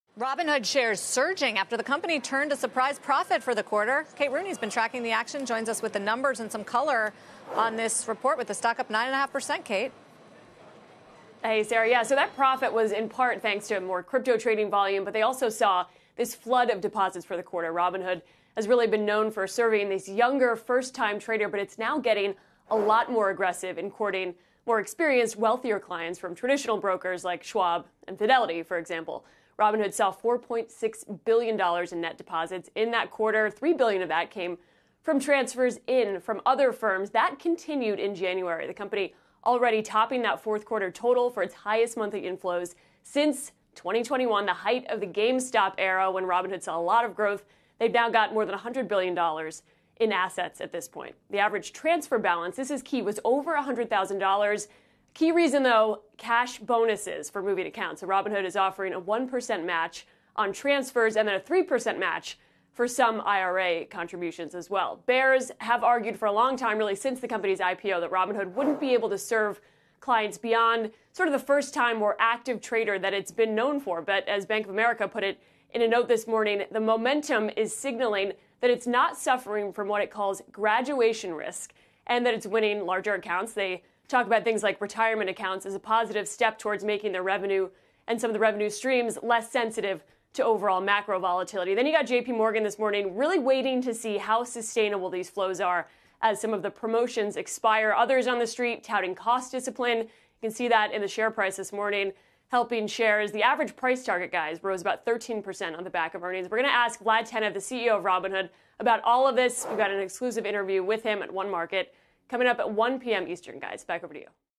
Robinhood: Here's why the stock is surging
CNBC's Kate Rooney reports on news from Robinhood.